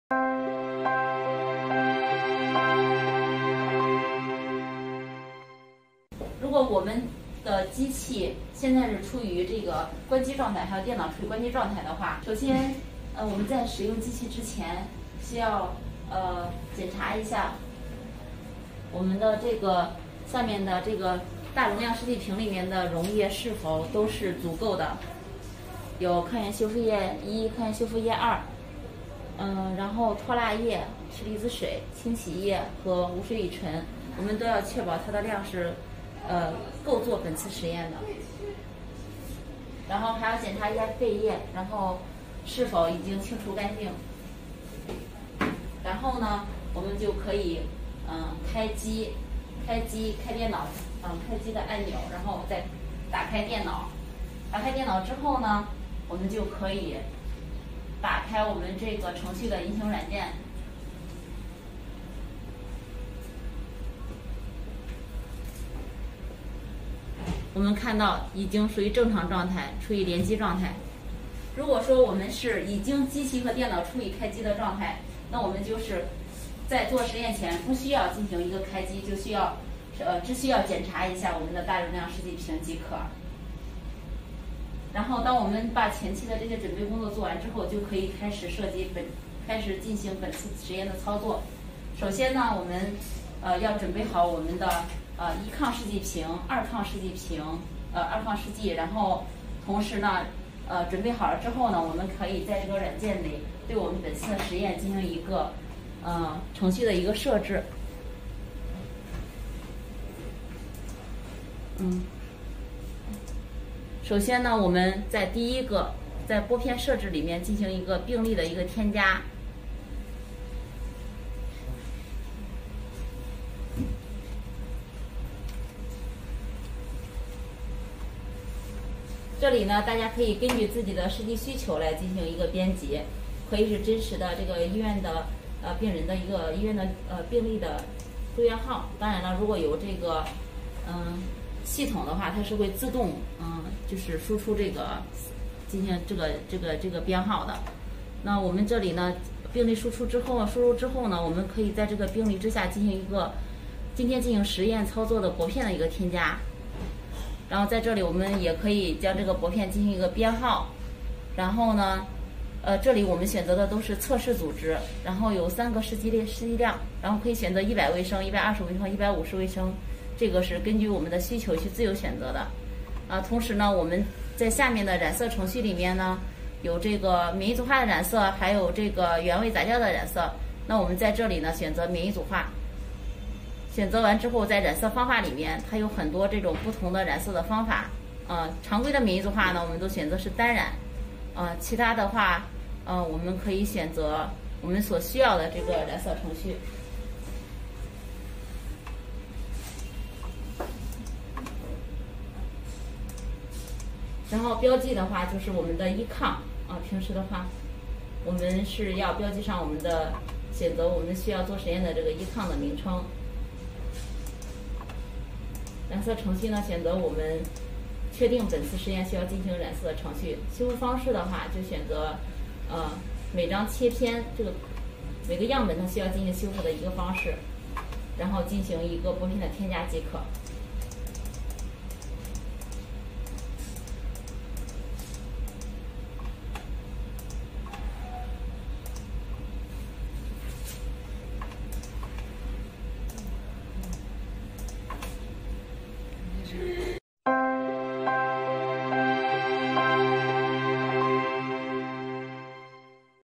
Bioevopeak Full Automatic Immunohistochemistry Stainer / IHC Stainer, IHCS-A30
Full Automatic Immunohistochemistry Stainer / IHC Stainer, IHCS-A30

Full Automatic System
Application: IHC Single Staining/CISH (EBER)
Run Time: 3~4 hours/user defined
Throughput: 30 slides loaded at the same time
Capacity of Reagent Tube: 7ml and 30 ml
Sustained Loading